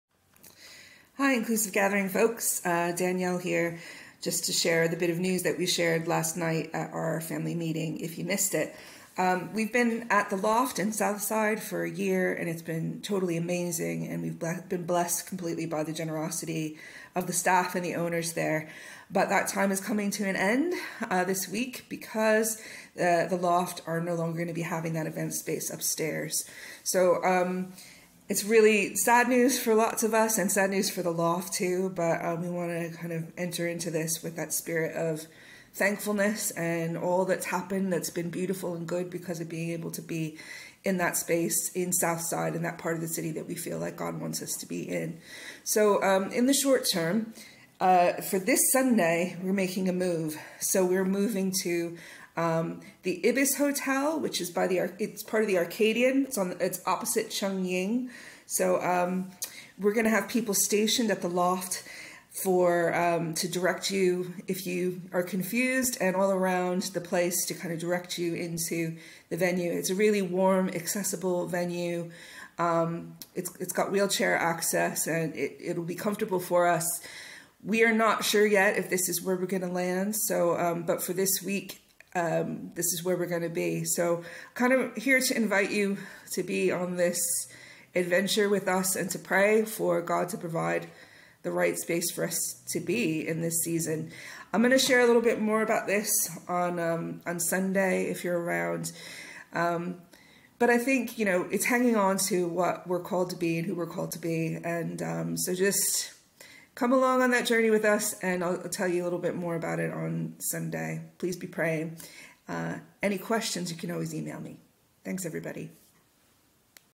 Big Announcement - IGB is moving!
Here's a very short video from our pastor Danielle if you missed our Family Meeting last night ... Our big news is that from THIS Sunday we will no longer be meeting at the Loft for our gatherings. (Danielle explains why in the video).

We are still in a time of transition, but for the month of September we will be meeting at the Ibis Hotel, 21 Ladywell Walk - Arcadian in the Southside District.  This is about a 2 minute walk from the Loft and a 5 minute walk from New Street Station.

In this season of change, it would be fab if you could join this Whatsapp Broadcast Community - we'll use it sparingly for announcements-only what's happening so we can update people quickly.  (Only admins will be able to see your phone numbers).

See you Sunday 3rd September at 3pm at the Ibis Hotel!